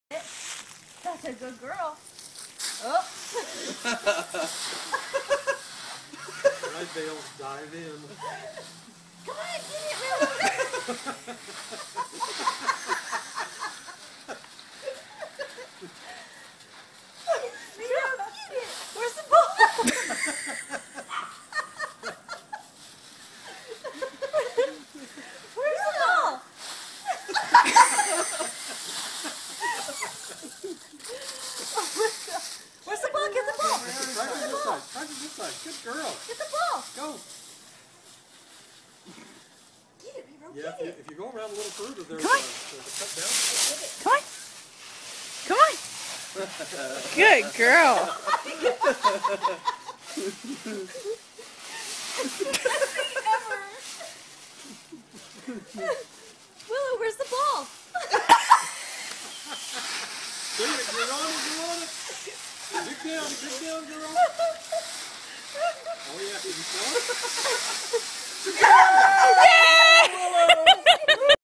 Can a tiny dog fetch a ball from a ginormous leaf pile?
It's like finding a needle in a haystack but unbearably adorable ...